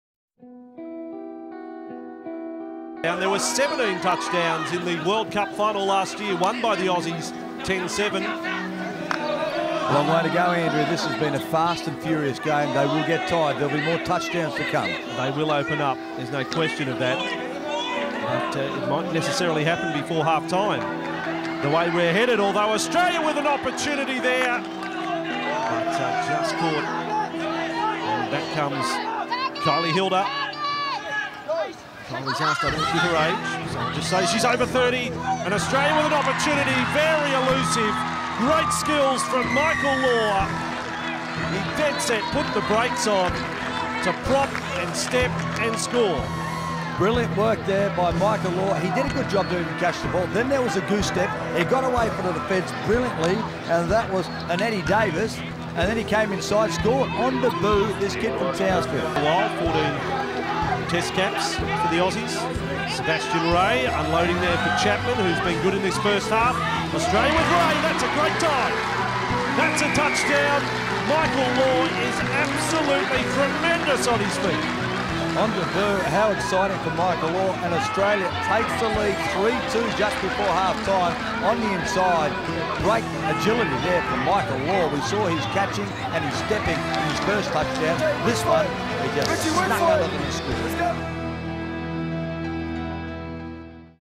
12 Plays of Christmas - #3 Law Impresses On Debut
Join us as we look back at 12 of our favourite plays of the year in the '12 Plays of Christmas' special.

Next up is Australian Mixed Open player, Michael Law's impressive debut at the 2012 Trans Tasman Series in Mudgee.